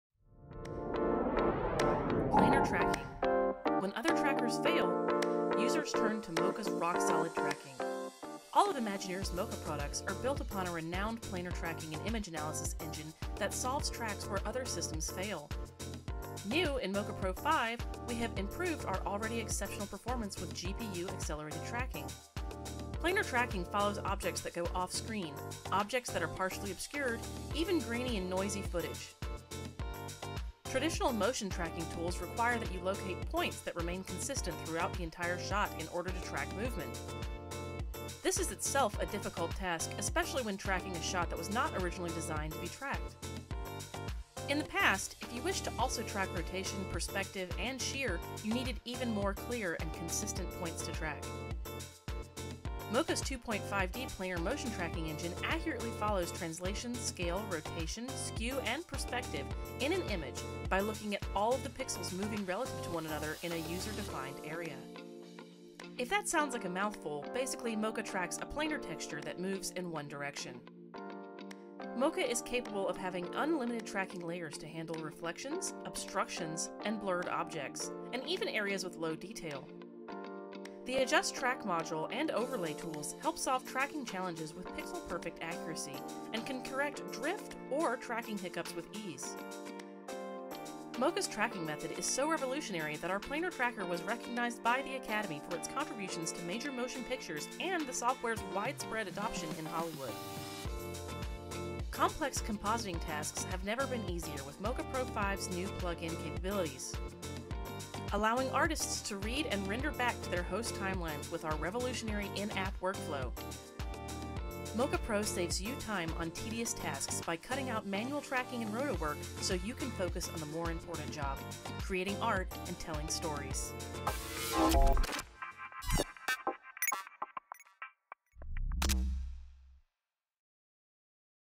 Mocha Pro: Planar Tracking
Mocha's Academy award-winning planar tracking engine accurately solves the most difficult motion tracking challenges. Unlimited tracking layers and advanced user interface can handle reflections, occluded and blurred objects, even areas with low detail.

New in mocha Pro 5, OpenCL GPU accelerated tracking gives significant speed enhancements on supported HW.